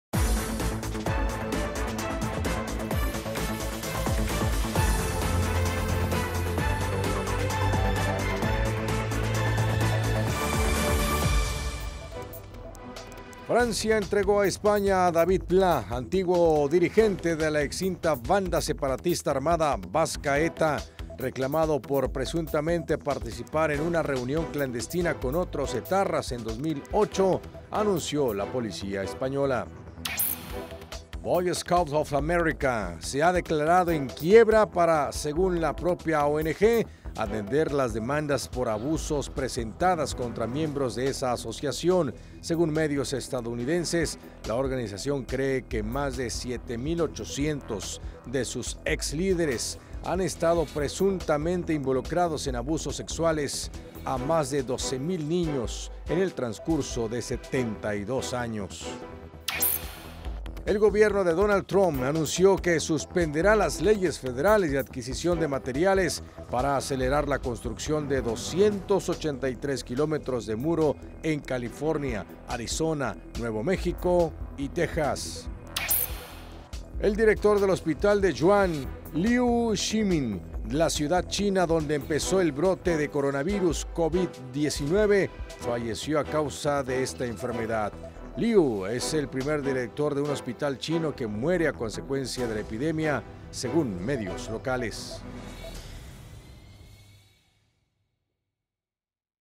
Internacionales.
Antiguo dirigente de ETA, es entregado a España. Infórmate en las internacionales.